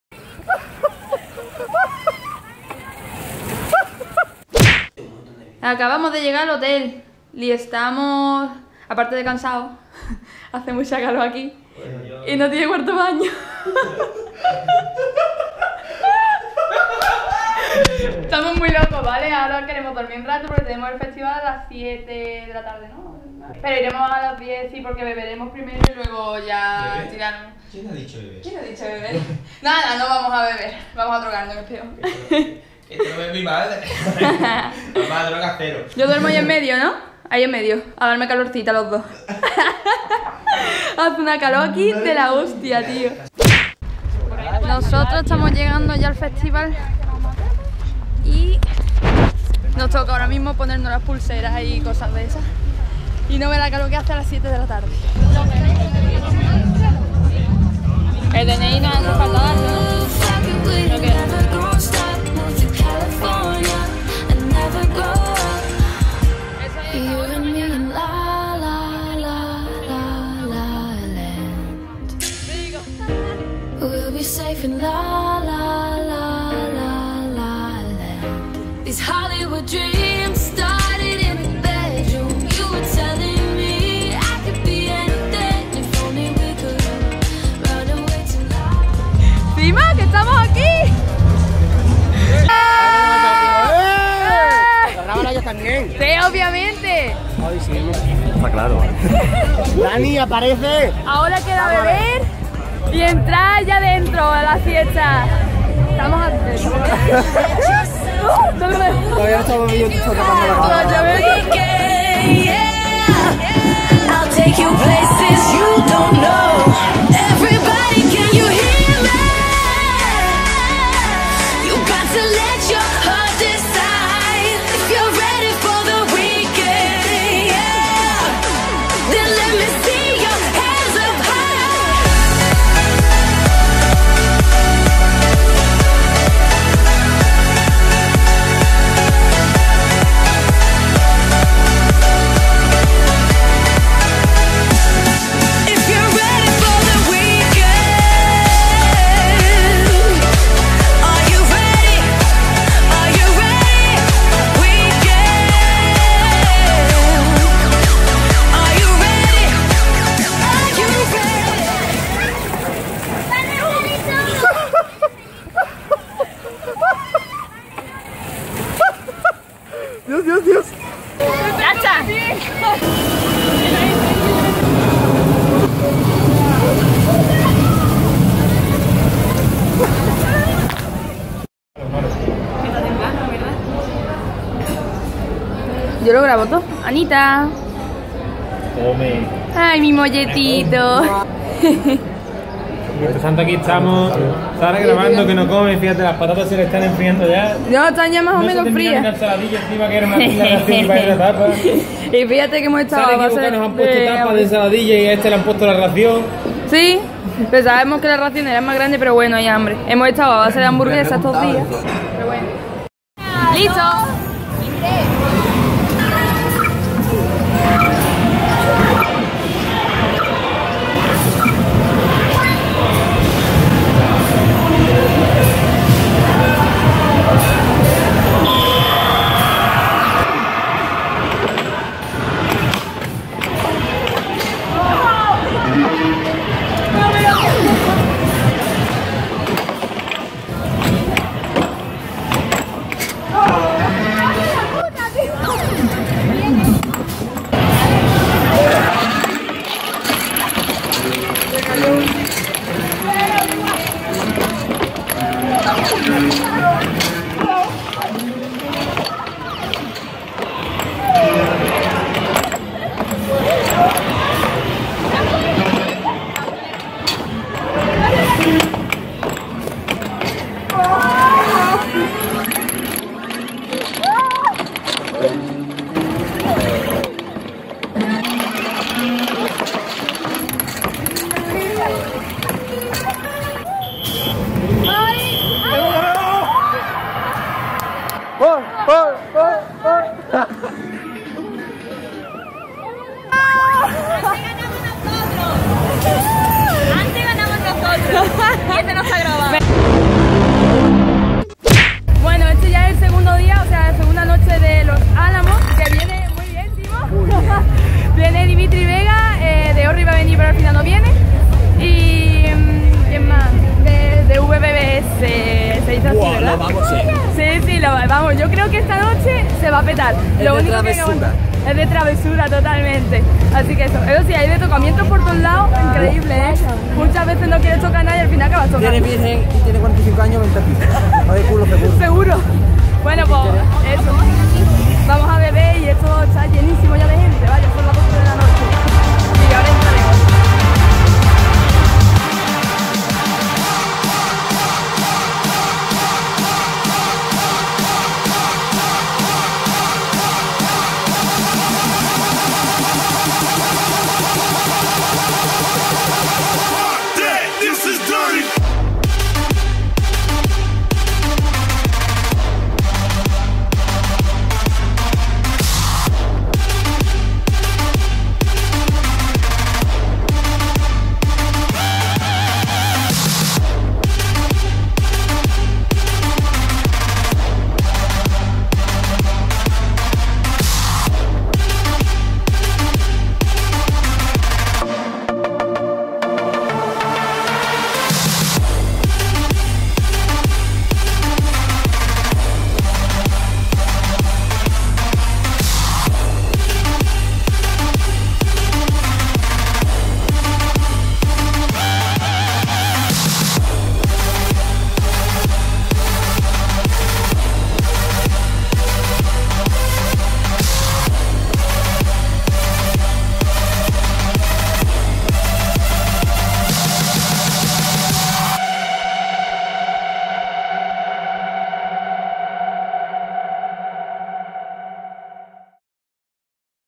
Los Alamos beach festival 2016 Videovlog , asi viví la fiesta :3
HOLAAAAAAAA pequeño video de toooodo lo que hice en malaga (los alamos) :3 disfrutad y apuntaros la proxima vez!

Salto a la movida MIN 1:53